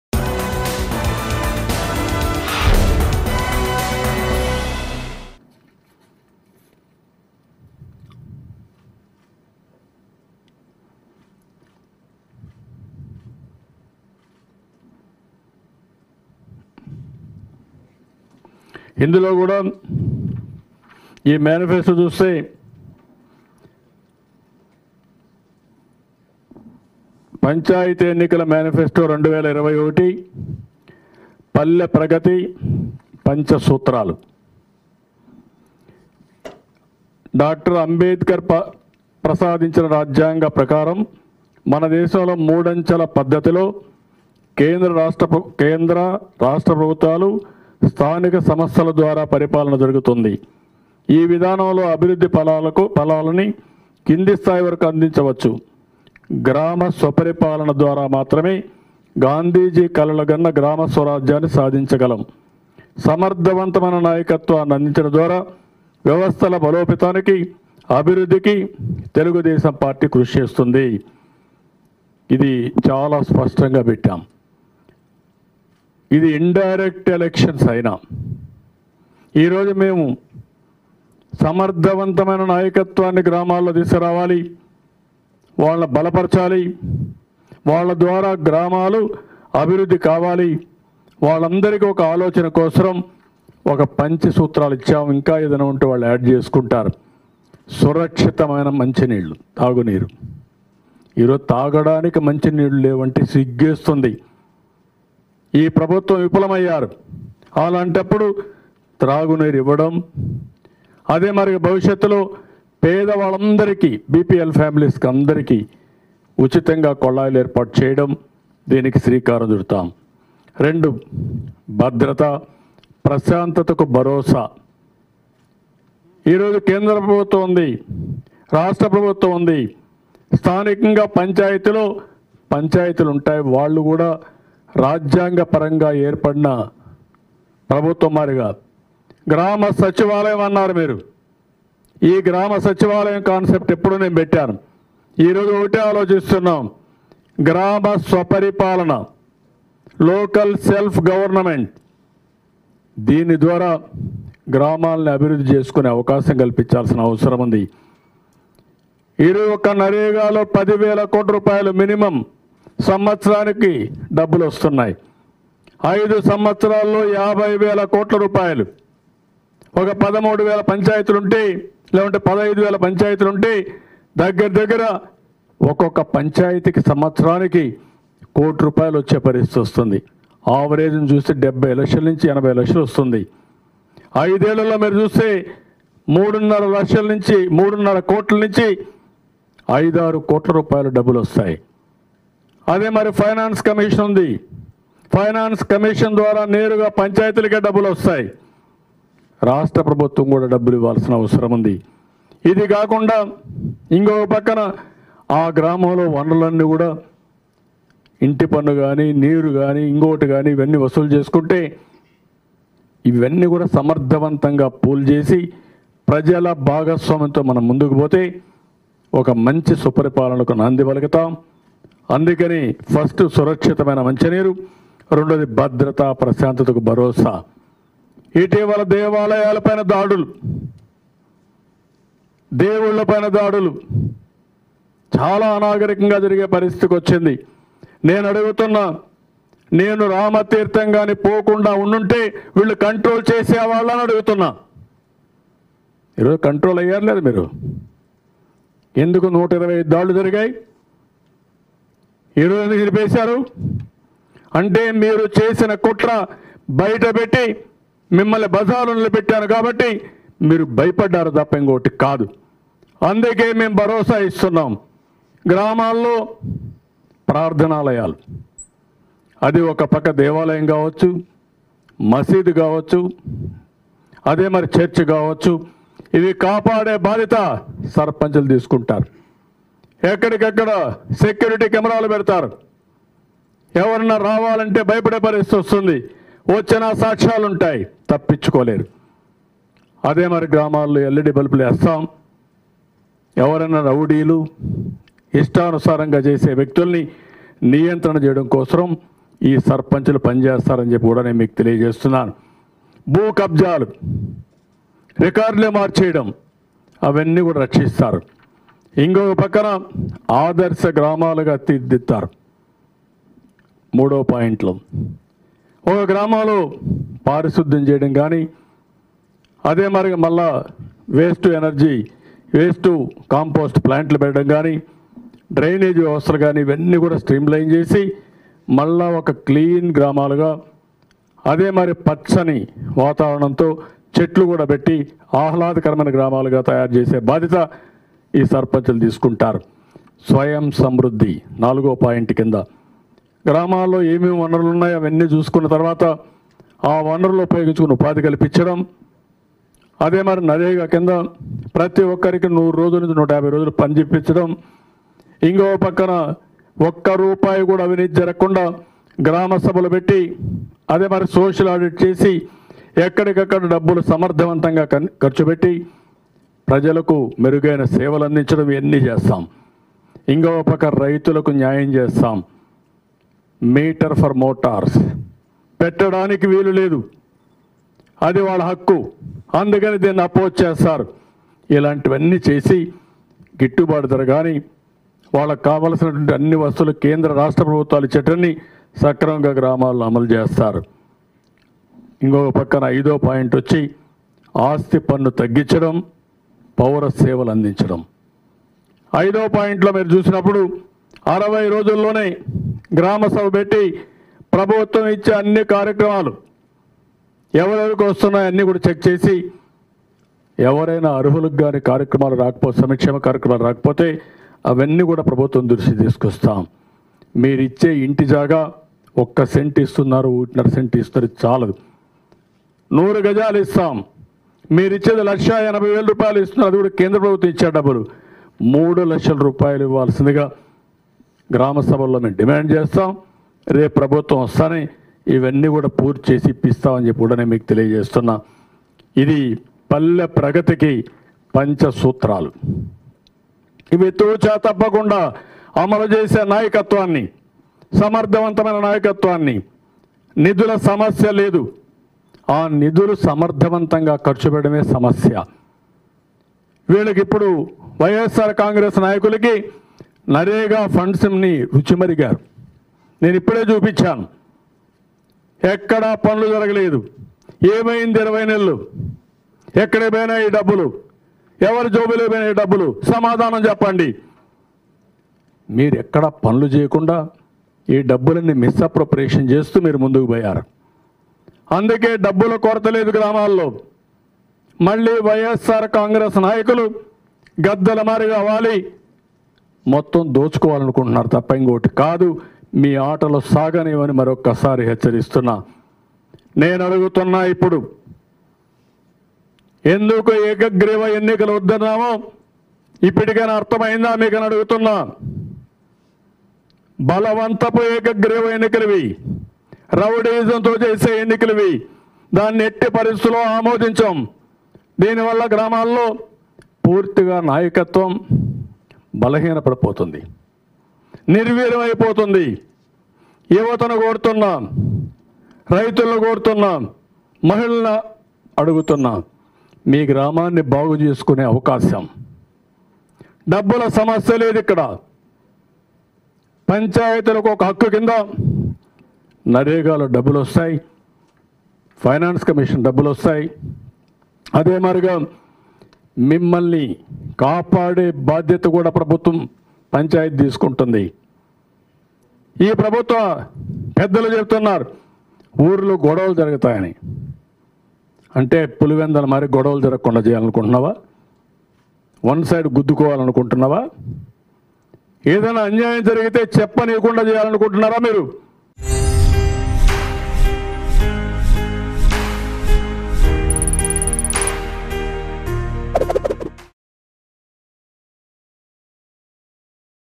Chandrababu Naidu Releases TDP Manifesto For AP Local Body Elections | AP Politics | Mango News
Chandrababu Naidu Releases TDP Manifesto For AP Local Body Elections.